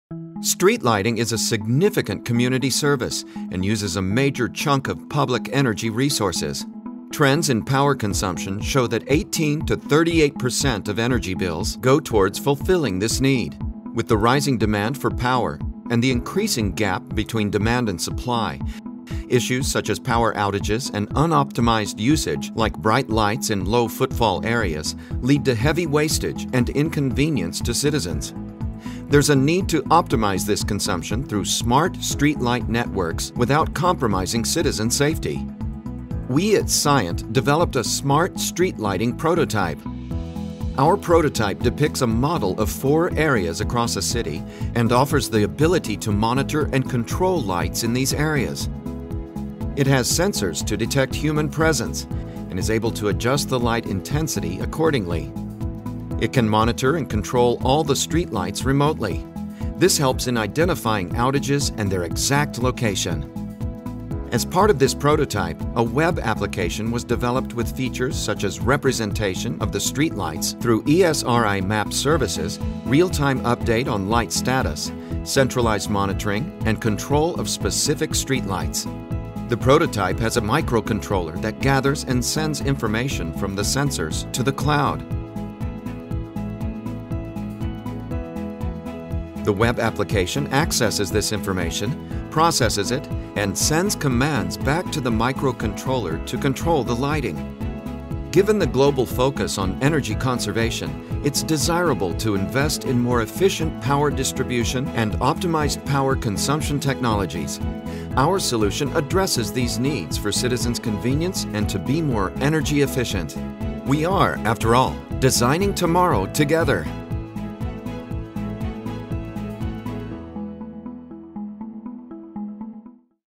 IoT-Based Smart Street Lighting Enhances Energy Conservation
Street lights are a mandatory community service requiring significant public energy resources. Understanding the need to optimize power consumption without compromising citizen safety, Cyient developed an IoT-based Smart Street Lighting prototype that can monitor and control lights remotely, detect human presence, and identify outages.